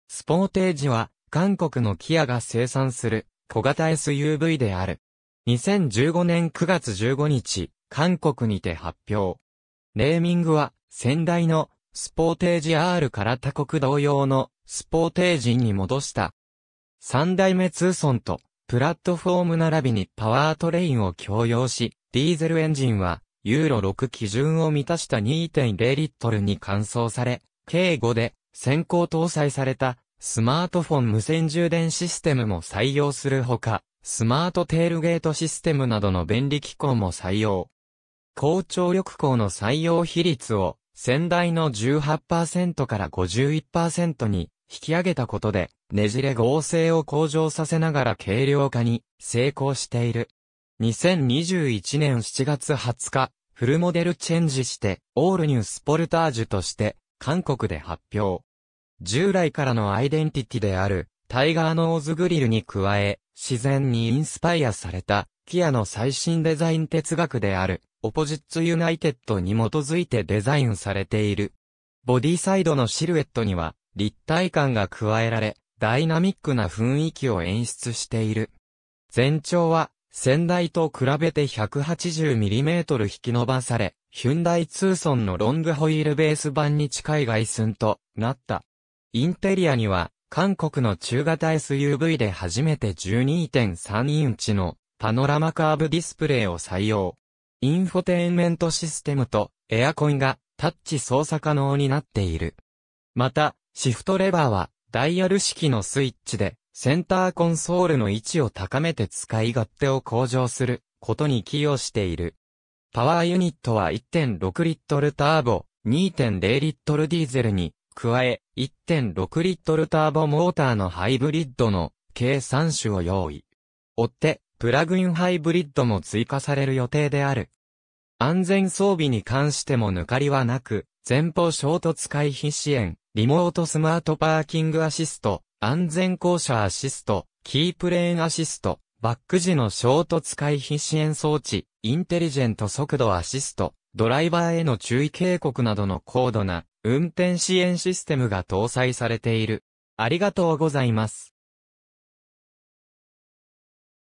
キア・スポーテージ
スポーテージ （Sportage ）は、韓国のKIAが生産する小型SUVである。
2015年9月15日、韓国にて発表。
ネーミングは先代の「スポーテージR」から他国同様の「スポーテージ」に戻した。
3代目（TL型）ツーソンとプラットフォームならびにパワートレインを共用し、ディーゼルエンジンは「ユーロ6」基準を満たした2.0Lに換装され、K5で先行搭載されたスマートフォン無線充電システムも採用するほか、スマートテールゲートシステムなどの便利機構も採用。
高張力鋼の採用比率を先代の18％から51％に引き上げたことで、ねじれ剛性を向上させながら軽量化に成功している。
2021年7月20日、フルモデルチェンジして「ALL NEW SPORTAGE」として韓国で発表。
従来からのアイデンティティである「タイガーノーズグリル」に加え、「自然」にインスパイアされたキアの最新デザイン哲学である「OPPOSITES UNITED」に基づいてデザインされている。
ボディサイドのシルエットには立体感が加えられ、ダイナミックな雰囲気を演出している。
全長は先代と比べて180㎜引き延ばされ、ヒュンダイ・ツーソンのロングホイールベース版に近い外寸となった（ホイールベースは2,755㎜で同一）。
インテリアには韓国の中型SUVで初めて12.3インチのパノラマカーブディスプレイを採用。
インフォテインメントシステムとエアコンがタッチ操作可能になっている。
また、シフトレバーはダイヤル式のスイッチで、センターコンソールの位置を高めて使い勝手を向上することに寄与している。
パワーユニットは1.6Lターボ、2.0Lディーゼルに加え、1.6Lターボ+モーターのハイブリッドの計3種を用意。
追って、プラグインハイブリッドも追加される予定である。
安全装備に関しても抜かりはなく、前方衝突回避支援、リモートスマートパーキングアシスト、安全降車アシスト、キープレーンアシスト、バック時の衝突回避支援装置、インテリジェント速度アシスト、ドライバーへの注意警告などの高度な運転支援システムが搭載されている。